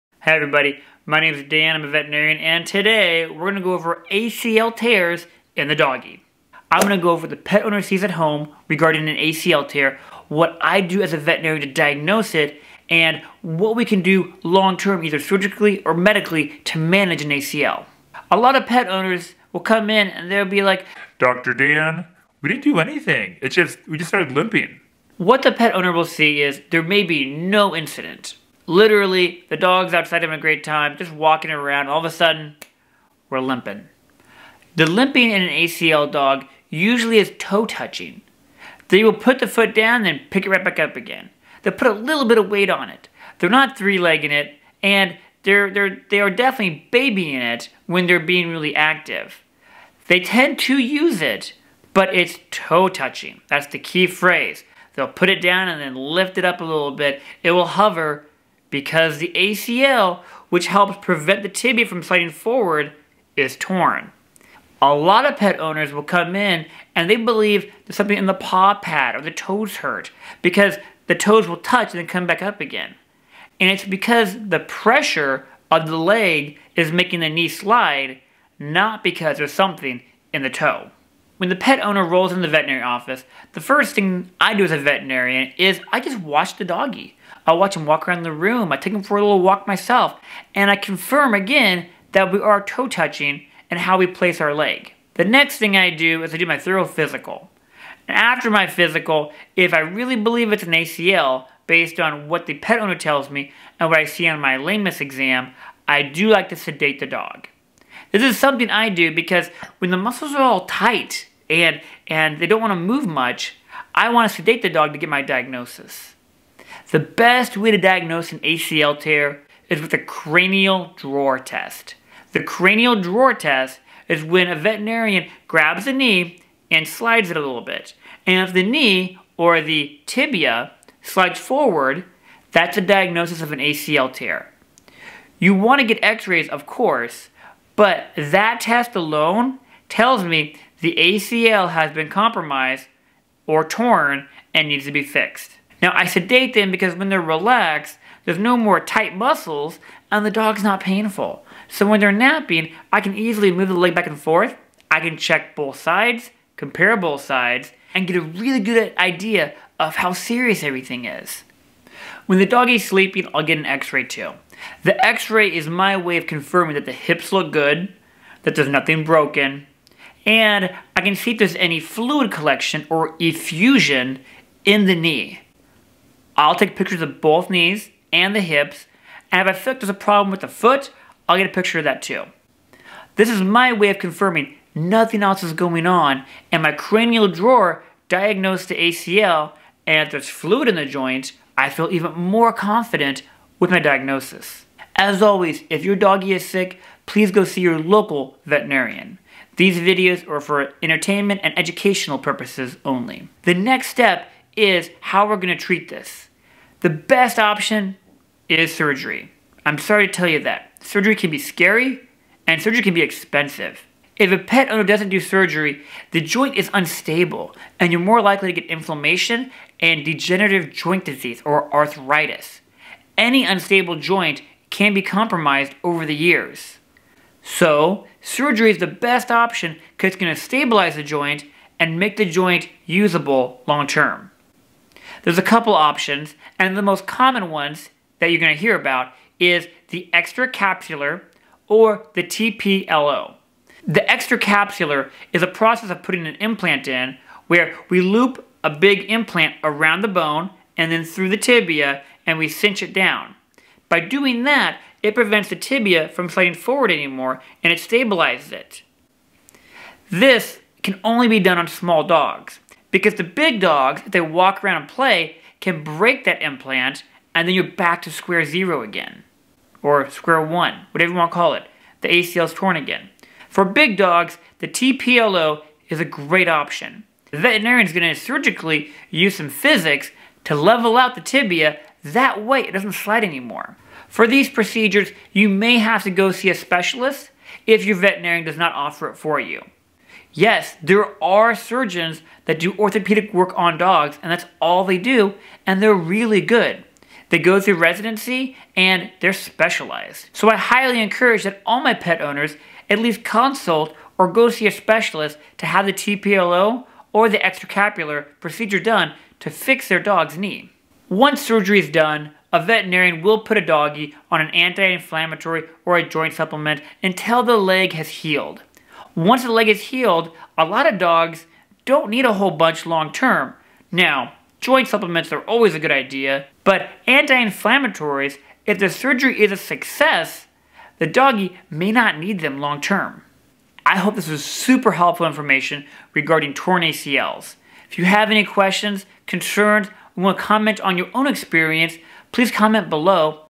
Dog ACL Tear! Dr. Dan explains why the ACL tears, symptoms, diagnosis, and medical or surgical
**This video is for entertainment and educational purposes only.  If your pet is sick, take them to see your local veterinarian for the care they needed** (Disclaimer)

Hey everyone!  My name is Dr. Dan and TODAY we are talking about the torn ACL in a dog.

As a veterinarian I see a lot of torn ACLs in dogs.  The dog ACL is the anterior cruciate ligament.  Your veterinarian may also call the ACL the CCL or cranial cruciate ligament.

When a dog tears the ACL the pet owner will notice a few things.  Sometimes a dog will tear their ACL during activity and sometimes it will just tear without trauma.  Some dogs will tear their ACL just because their ACL is not a strong or healthy as it should be and it just tears from daily use.

When the dog tears his/her ACL the pet owner will notice toe touching.  Toe touching is when a dog will put just a little weight on the back leg with the bad ACL.  When an ACL tears the stifle or knee joint is not stable any longer, so the dog will not want to put much weight on the torn ACL leg.

A veterinarian will diagnose a torn ACL in a dog by testing the cranial drawer of the knee or stifle joint.  If the dog is painful, the veterinarian may give the dog some sedation to test the ACL.  The cranial drawer test is positive if the tibia slides forward or cranially.  A veterinarian will also take x-rays to look for effusion or fluid in the knee or stifle joint.

The best way to treat an ACL tear is with surgery.  Surgery for big dogs includes a TPLO and small dogs usually get an extra cap surgery done.  A veterinarian will decide what is the best option.  An NSAID until surgery will help keep the dog less painful until surgery.